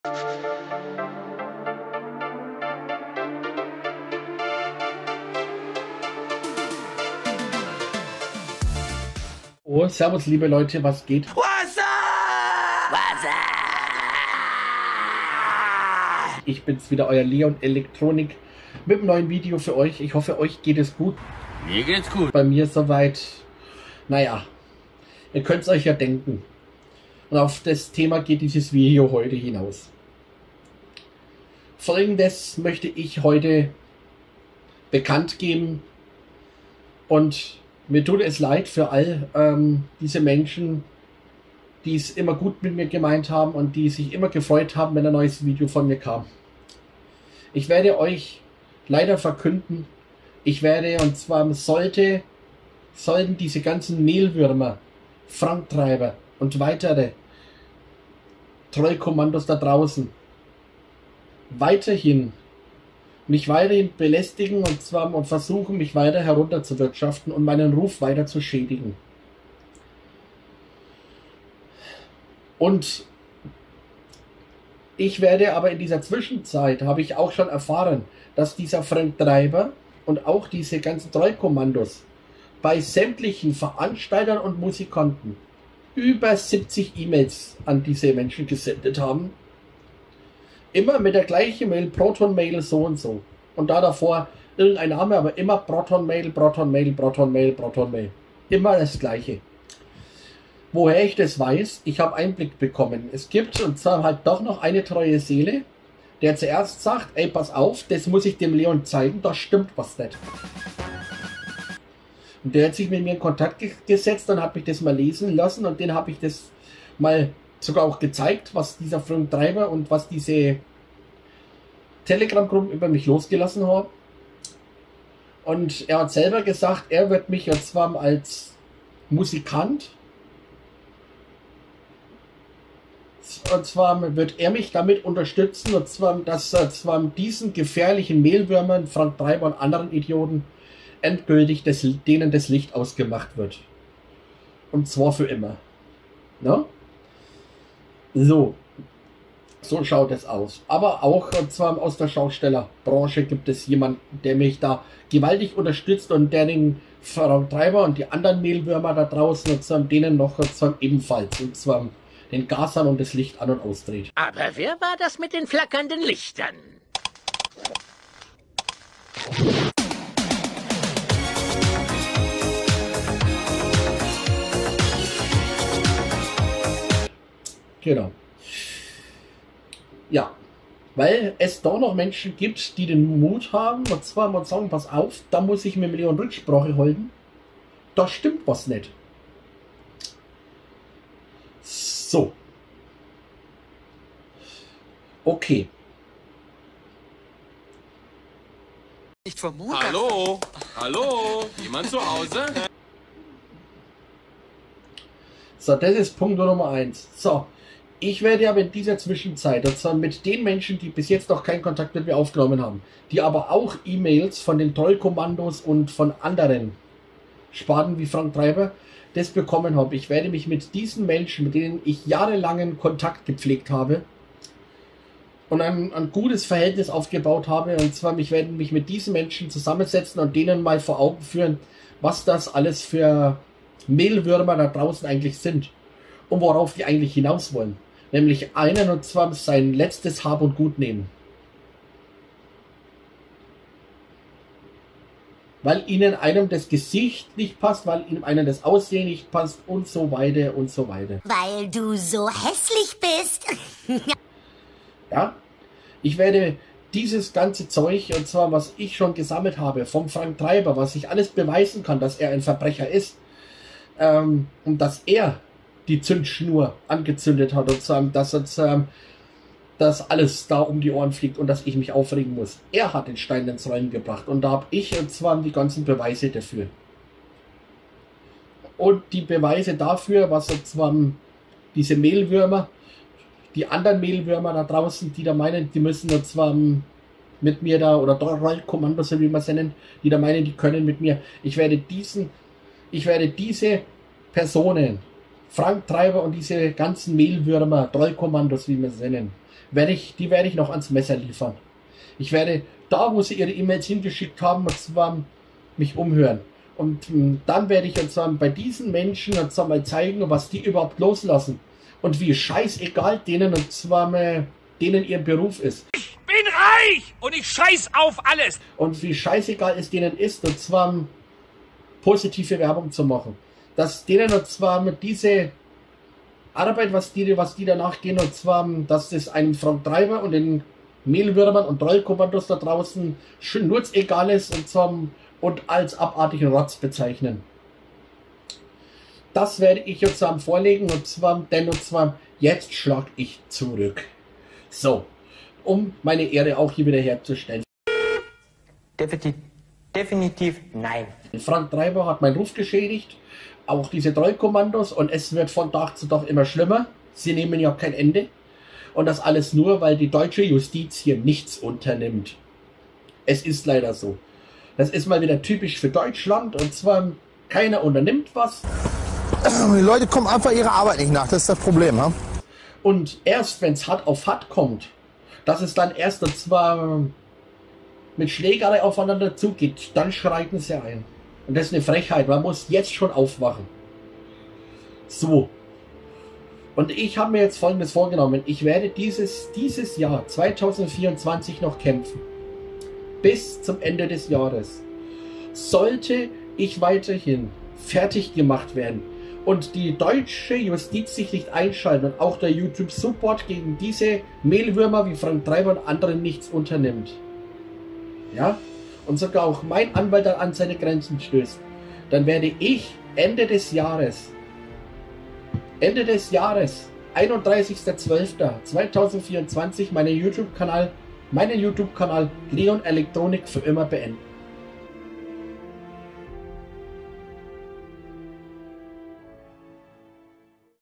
Leon Elektronik WICHTIGE Info!
Der gute Leon erzählt uns in diesem Video über seine Pläne für dieses Jahr. Er wird weiterhin gegen die Haiders vorgehen und stellt ein Ultimatum!

WICHTIG:
§ 24.1 UrhG: Ein selbständiges Werk, das in freier Benutzung des Werkes eines anderen geschaffen worden ist,
darf ohne Zustimmung des Urhebers des benutzten Werkes veröffentlicht und verwertet werden.